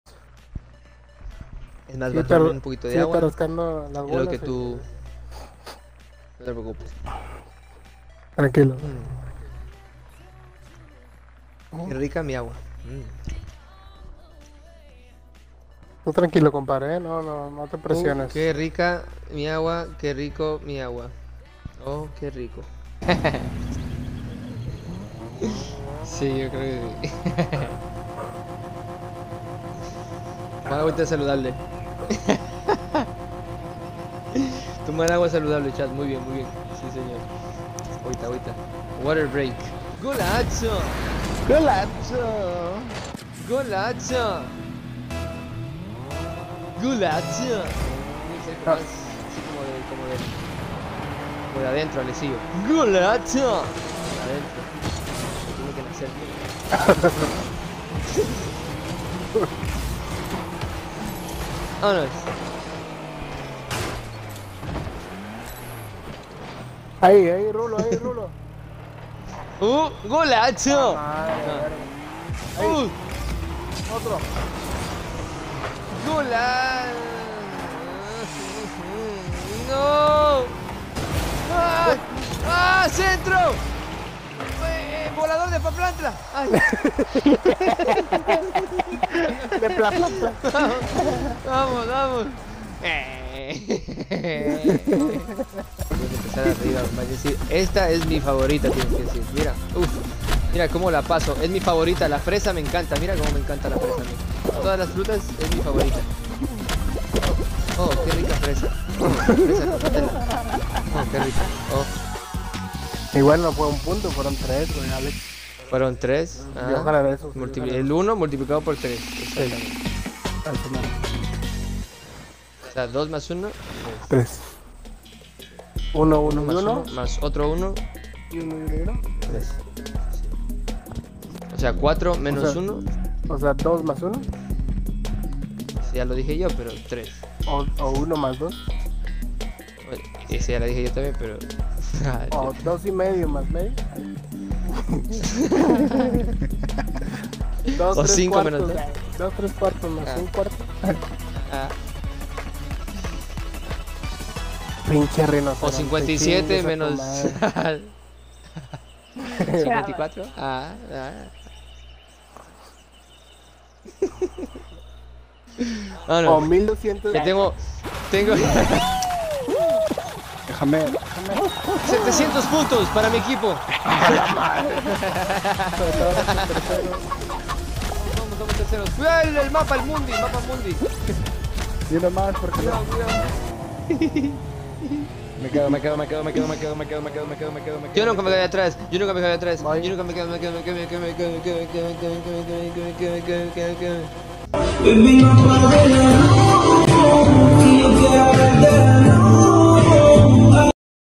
Clips de Chris por Twitch Anoche💗 | 7/5/2022
(estos son todos los clips que hice anoche, decidi ponerlos todos en un solo video en vez de subirlos aparte☺️)